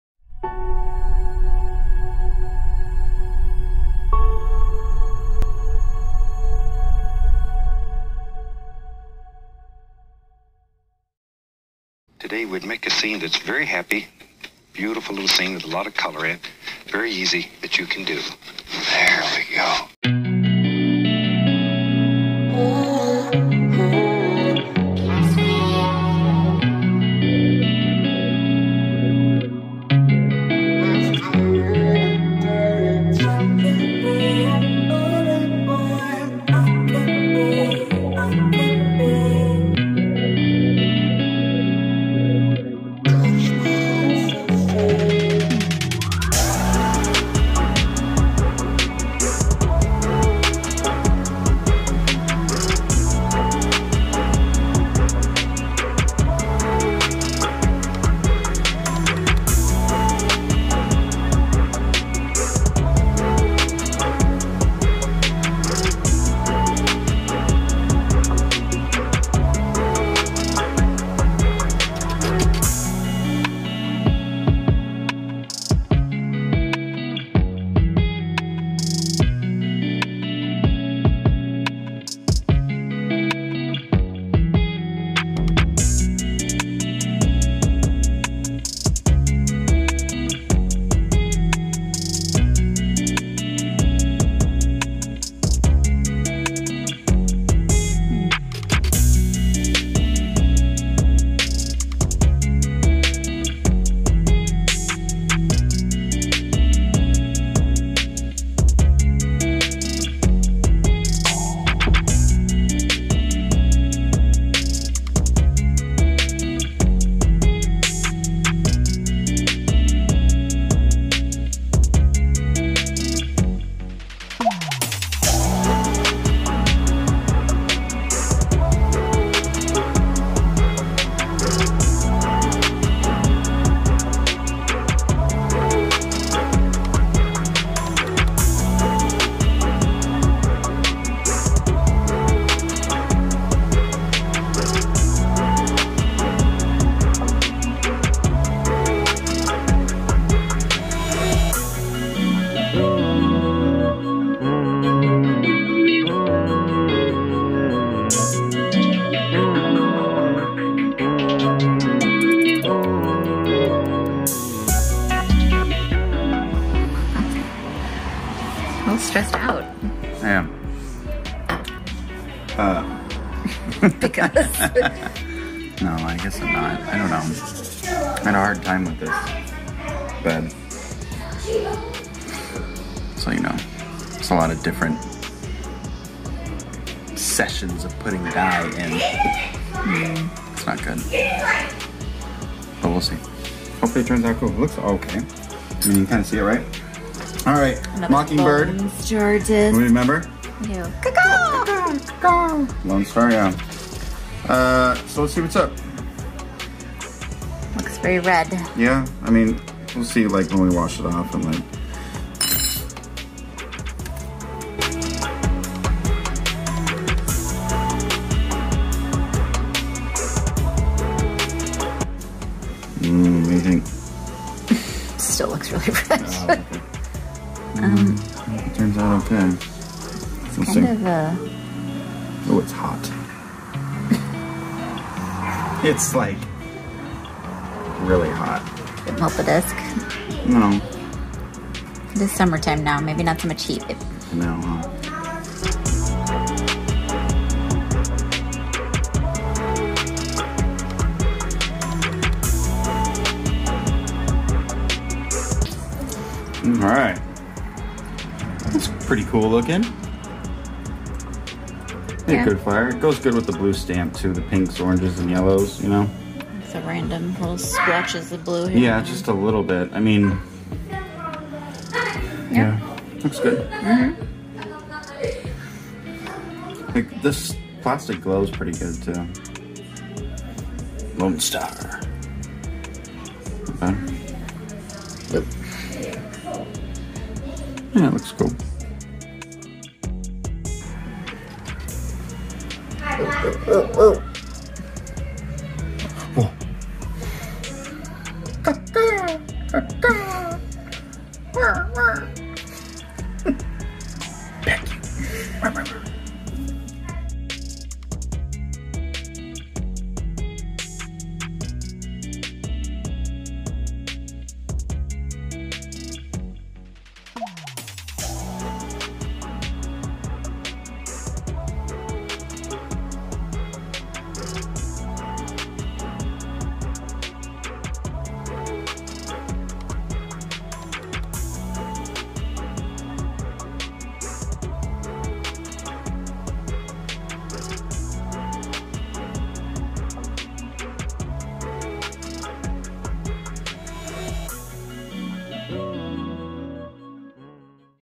#159 Dyeing a Glow Mockingbird from Lone Star Discs! Disc Golf Dye
Dyed up a Glow Mockingbird from Lone Star Discs! Check it out!